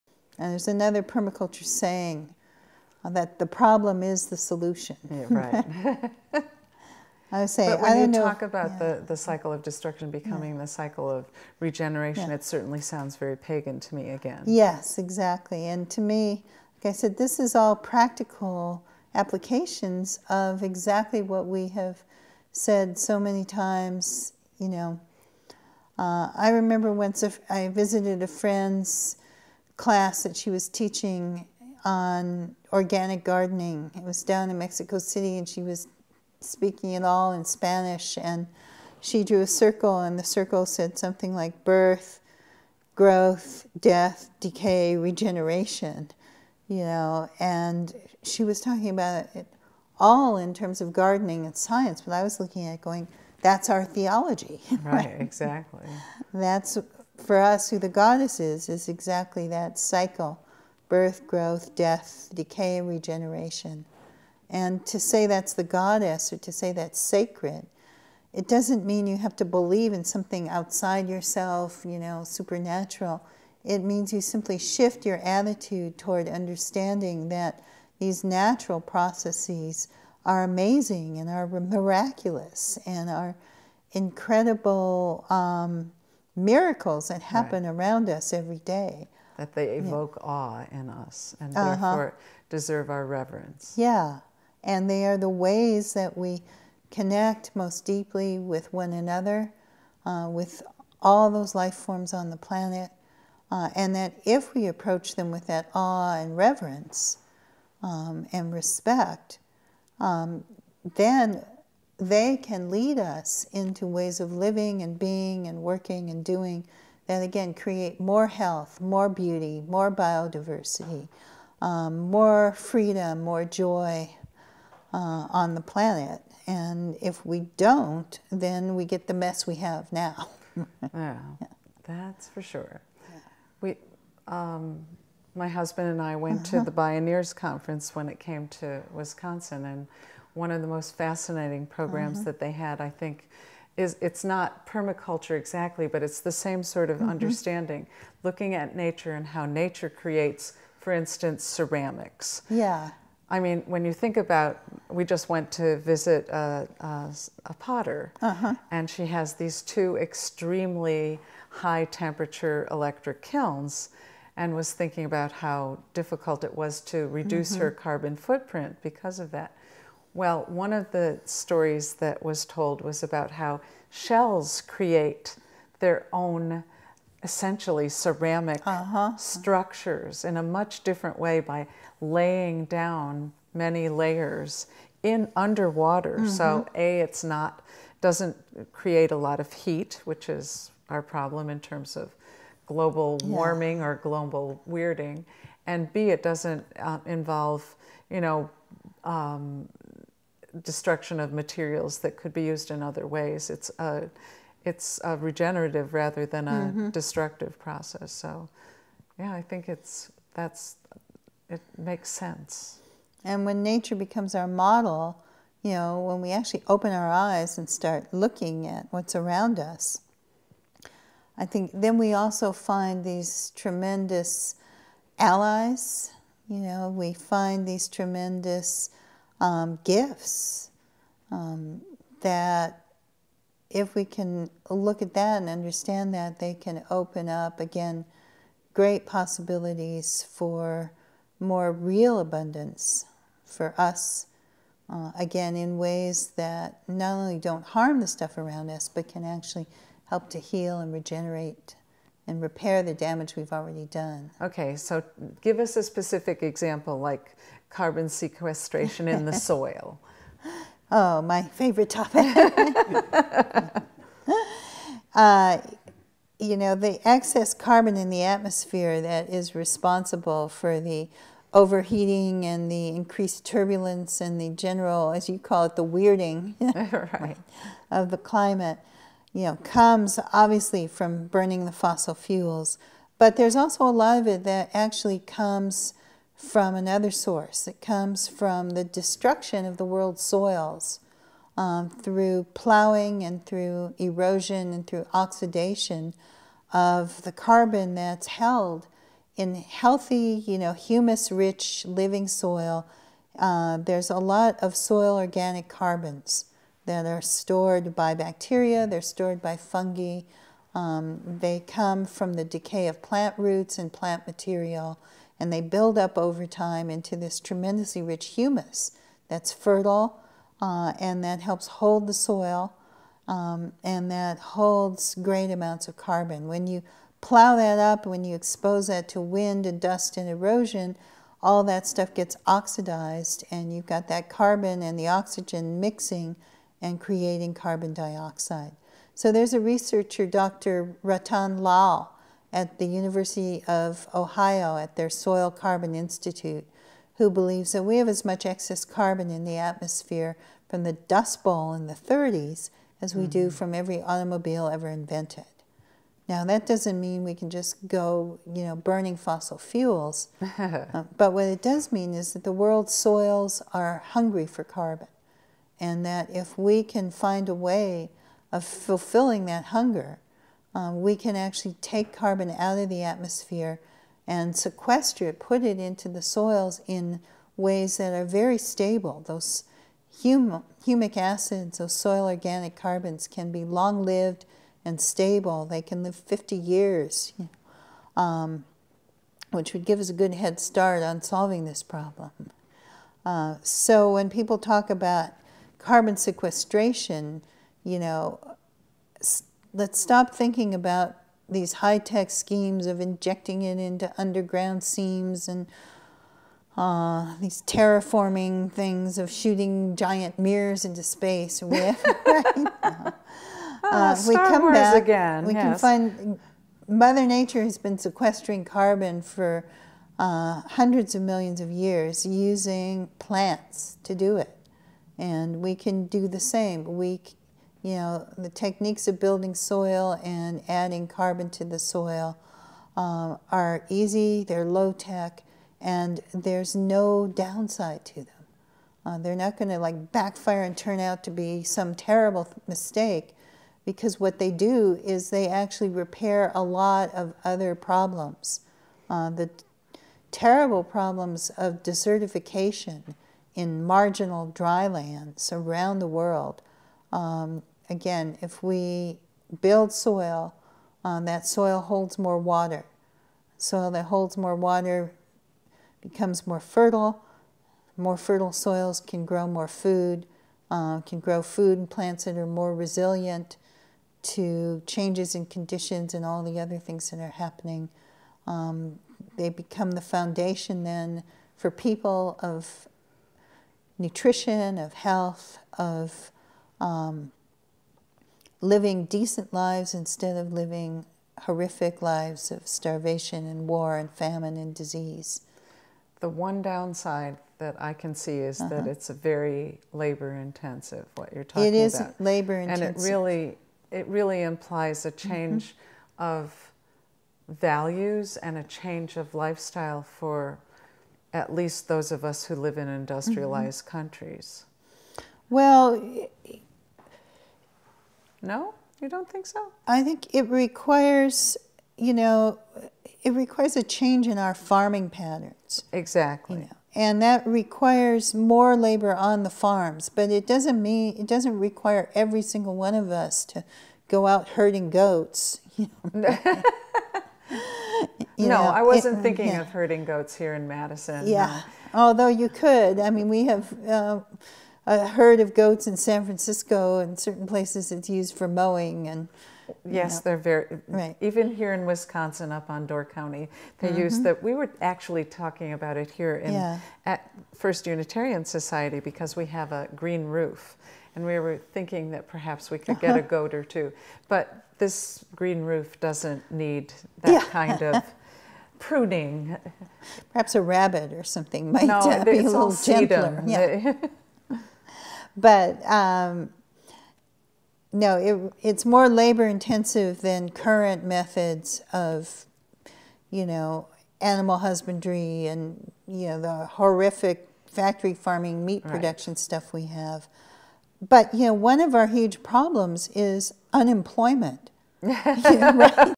Starhawk5 an Interview
Author / Activist Starhawk speack with Nancy Vedder- Shults about Palestine , Permaculture and Paganism part 5 of 6